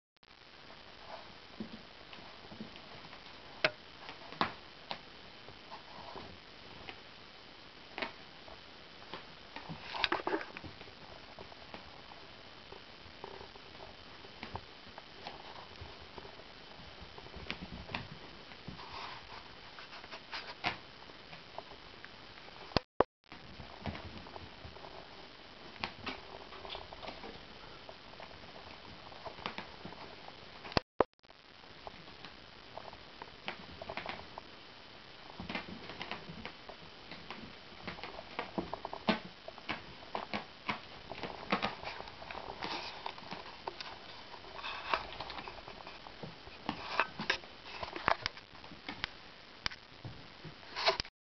mój perski kotek. Gatto persiano .Cat
Stefan baraszkuje przed lustrem.